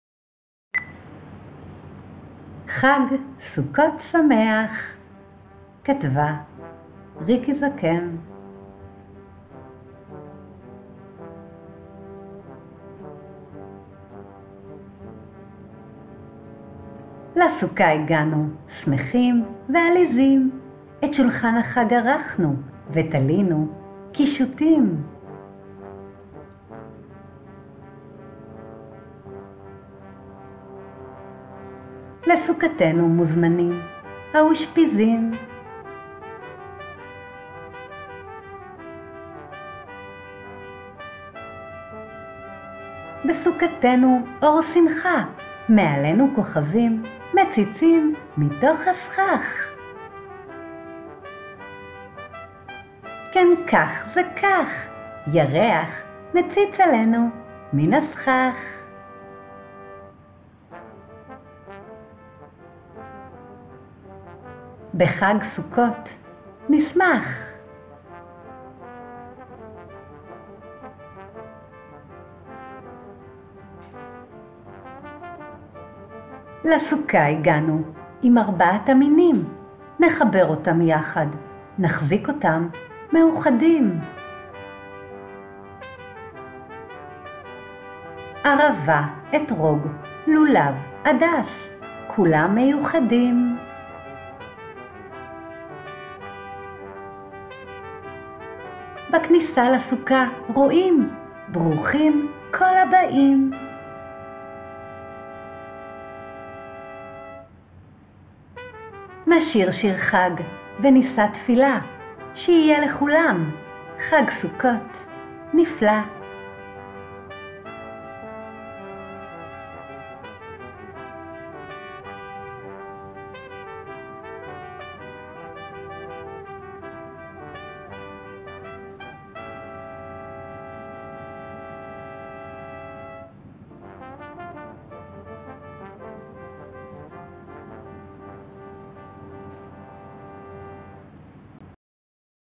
חג סוכות שמח - סיפוריקי
חג סוכות שמח - סיפור ילדים
כותבת יוצרת: ריקי זקן
לַסֻּכָּה הִגַּעְנוּ
שְׂמֵחִים וְעַלִּיזִים
אֶת שֻׁלְחָן הַחַג
עָרַכְנוּ
וְתָלִינוּ קִשּׁוּטִים